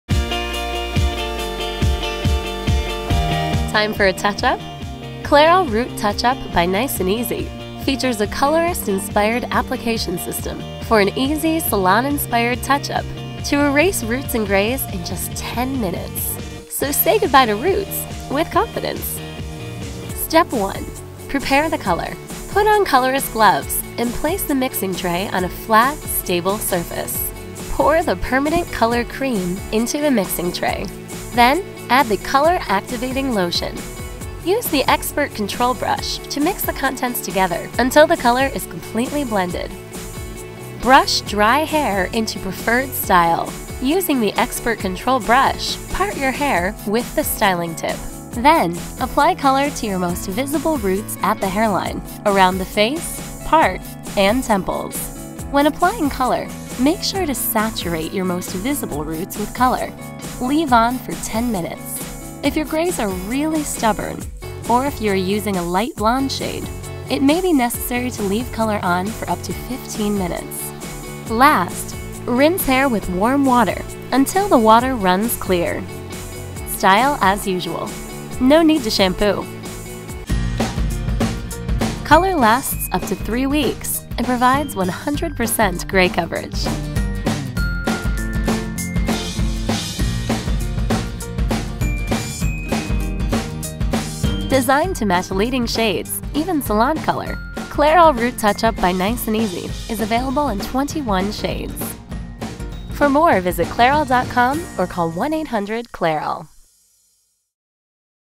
How to Do a Root Touch-Up with Nice 'n Easy | Clairol
Don't have time to stop by the salon? Are your roots growing in or grays coming back? Watch this step by step hair color tutorial on how to do a root touch-up with Nice 'n Easy at home.

This Clairol video is about using Nice 'n Easy to do a Root Touch-Up.